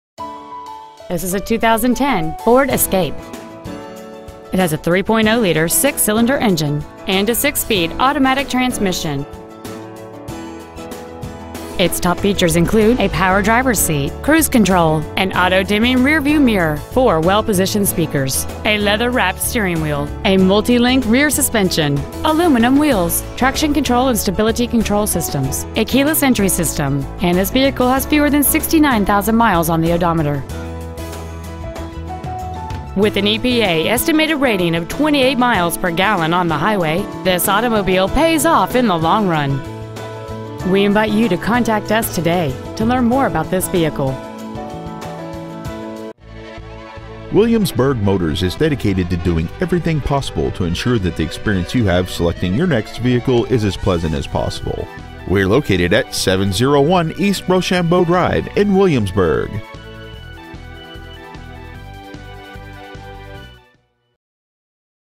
2010 FORD ESCAPE Williamsburg VA
We have been honored to serve the Williamsburg VA area , we promise that your experience at our dealership will exceed your expectations !

 Year : 2010
 Make : FORD
 Model : ESCAPE
 Engine : 3.0L V6
 Trans . : AUTO 6SPD
 Exterior : GRAY
 Miles : 68,377

 Stock : P877A